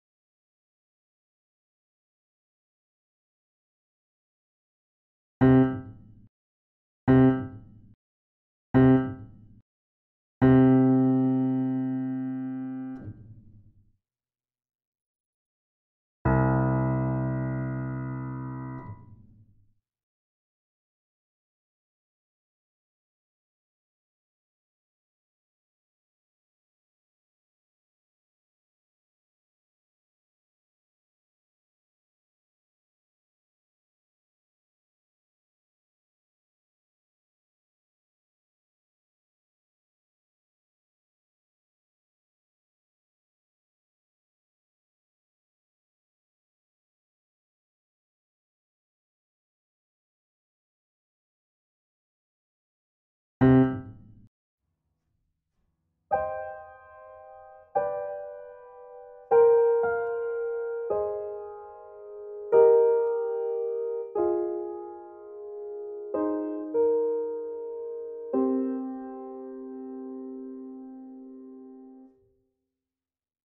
Upright Piano Action - Animated
3D animation of an upright action mechanism. Also included in de video is a schematic of all the parts making up the action. The action is based on a Yamaha B2 upright piano.
Animation and rendering done in Cinema 4D
Sound and music created in Reason